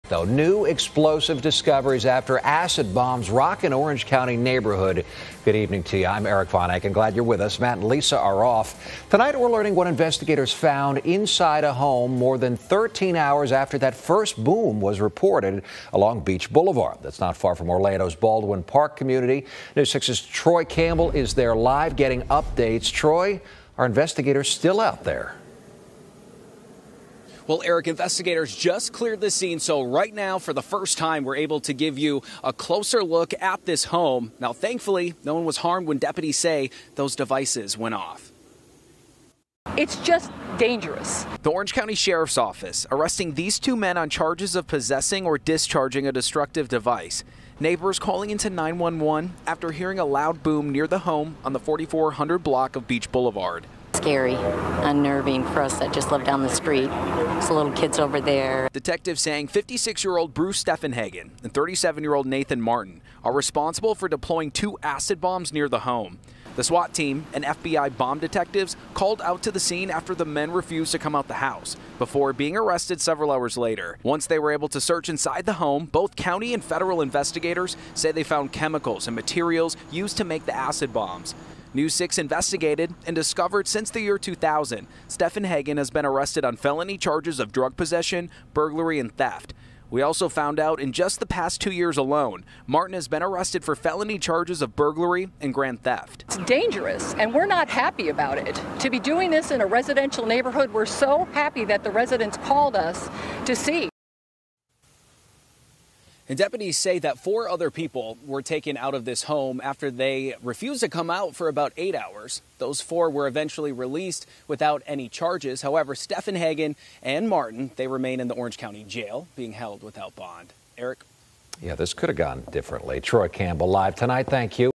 Scene cleared after acid bombs explode
An Orange county scene has been cleared after acid bombs exploded in the area.